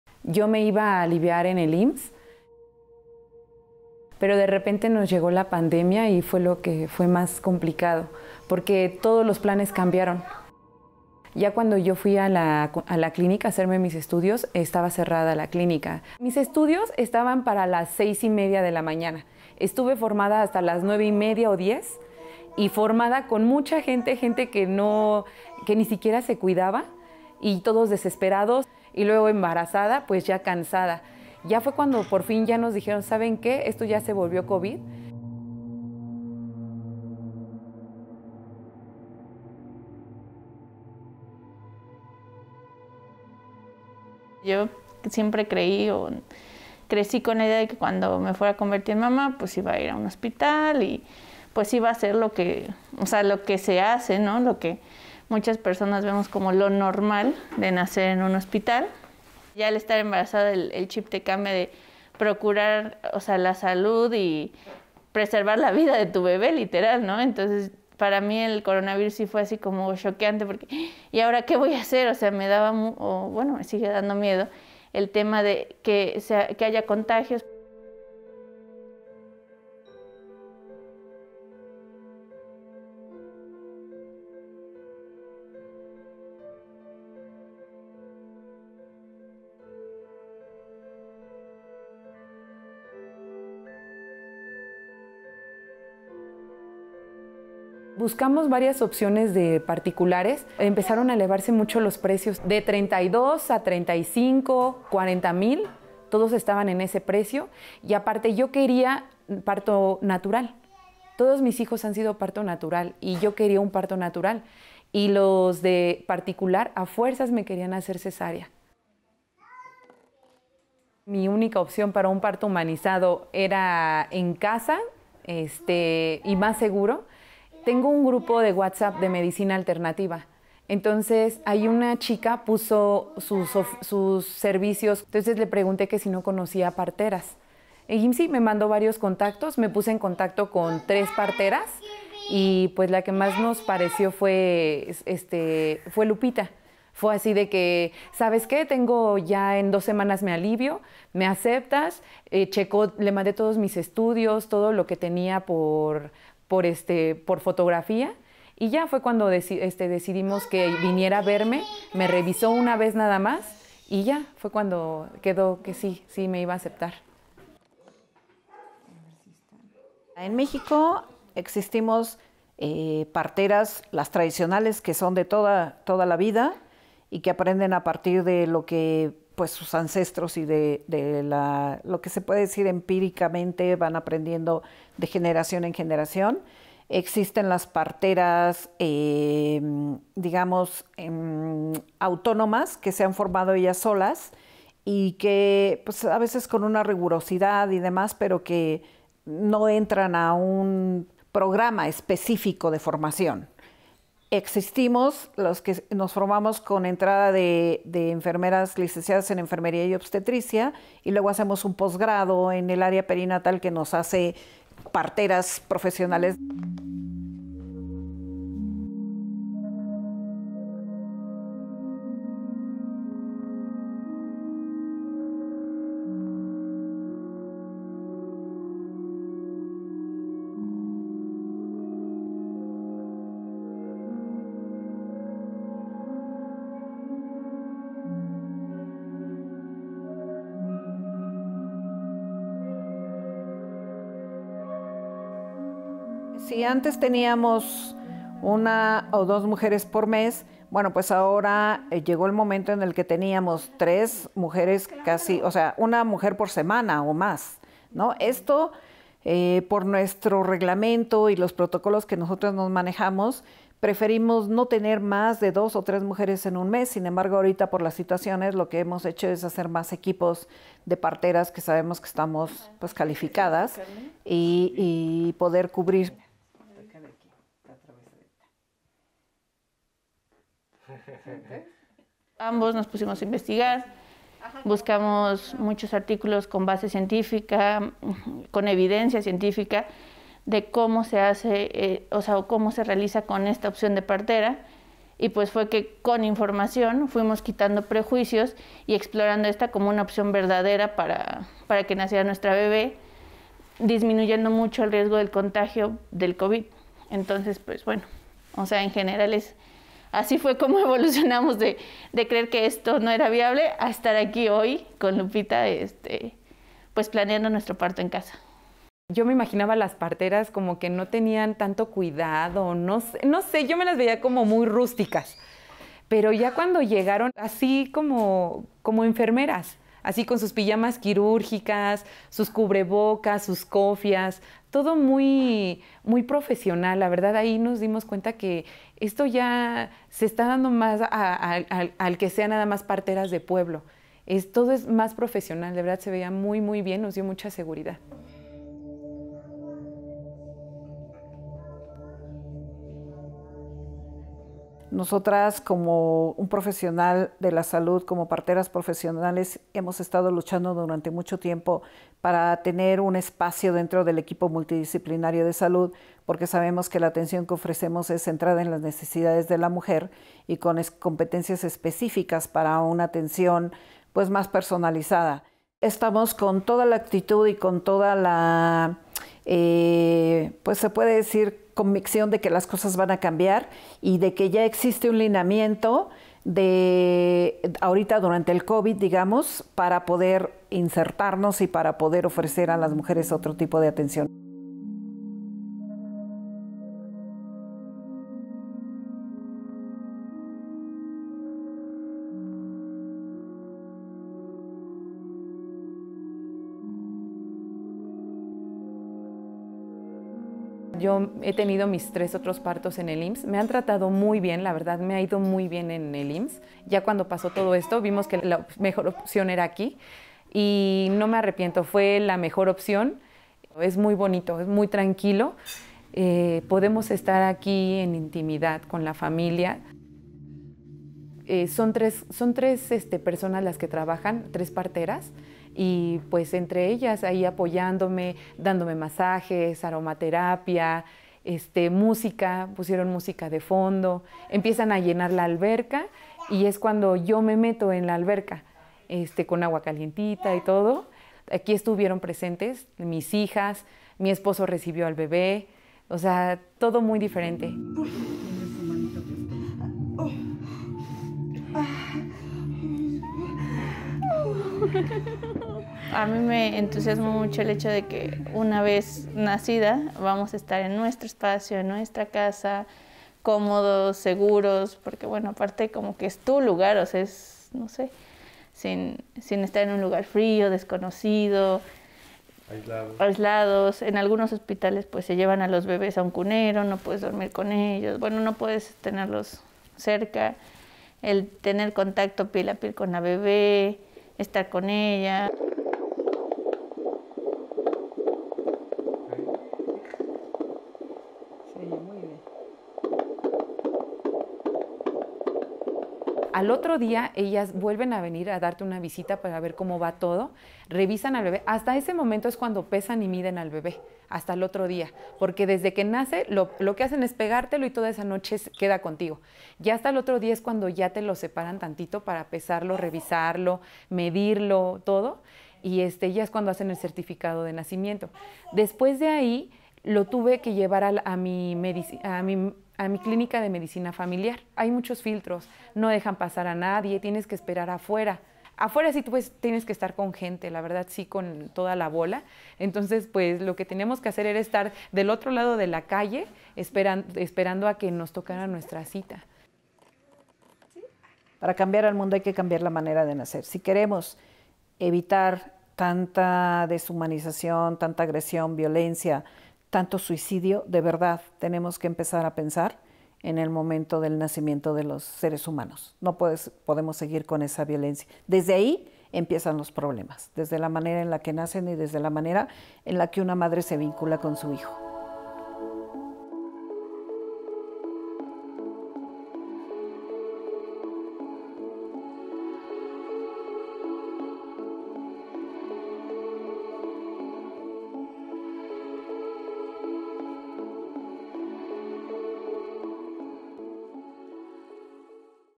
Parir en casa: una esperanza de vida en tiempos de COVID-19
Para atender a mujeres embarazadas durante la pandemia, los servicios de atención materna fueron considerados esenciales y se definió que no debían suspenderse, también se firmó un acuerdo con hospitales privados para atenderlas, pese a ello, la tasa de mortalidad materna aumentó. Ante el miedo al contagio y la  falta de información, muchas mujeres decidieron parir en casa.

¿Quieres ver más historias en video?

Historias, contexto y la información que necesitas para entender tu día a día en México